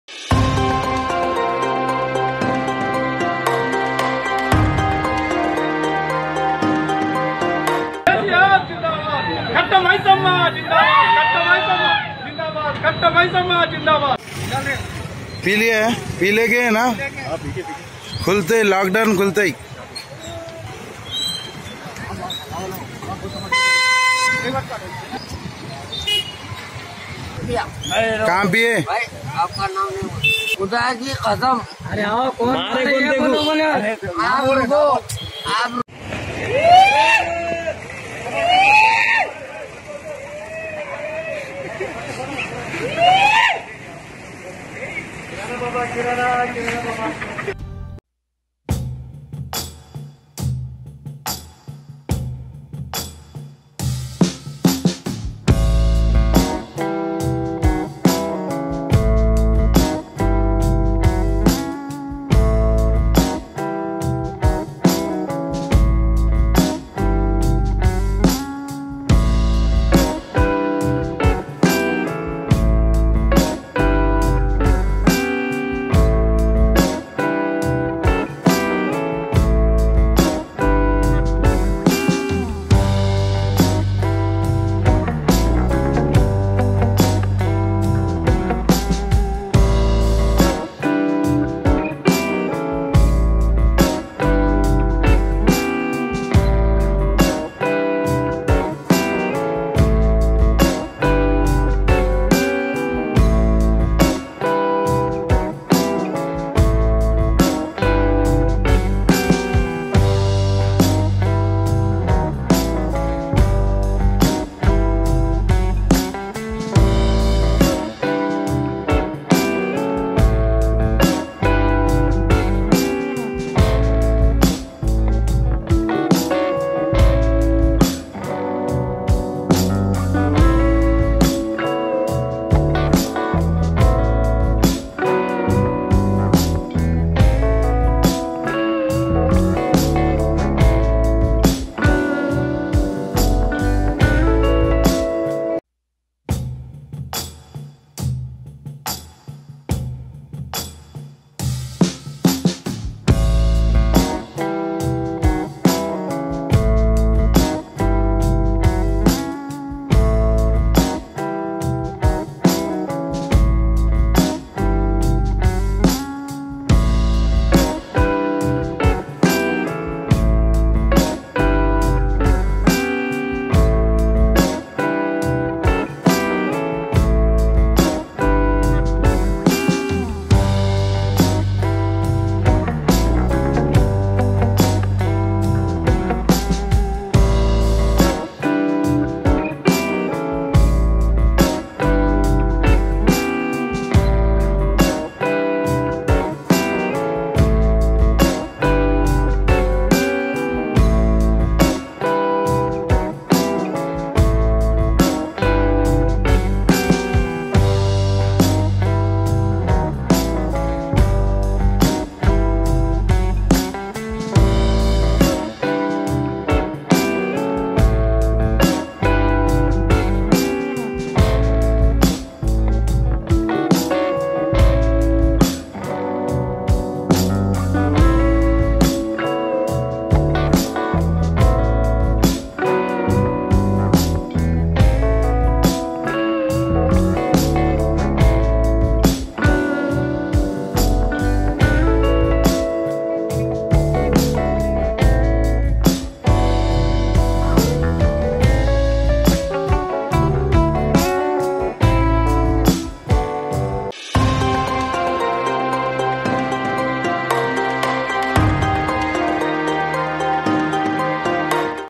మద్యం కోసం మహిళల పాట్లు - Women In Queue at Wine Shops | Girls in Liquor Que After LockDown | MAP TV
మద్యం కోసం మహిళల పాట్లు - Women In Queue at Wine Shops | Girls in Liquor Que After LockDown | Women Rush at Wine Shops